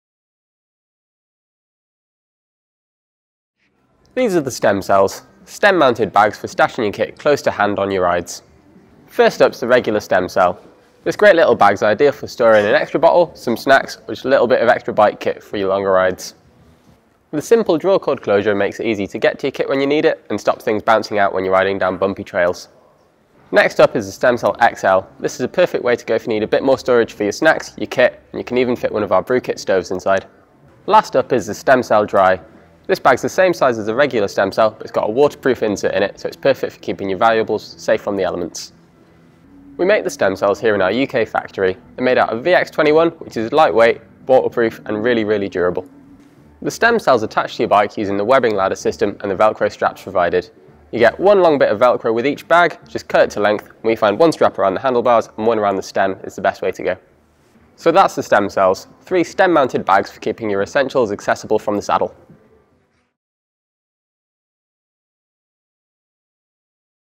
Product Insights: Stem Cells
Please note that  Stem Cell has now been replaced by the Stem Cell 2

Refuelling with an energy gel, taking a swig of water, capturing a sunset on camera… Some things just can’t wait until you hunka down for the night.

Stem Cell provides easy-to-access, protective storage space for the stuff that you’ll need frequently during your ride.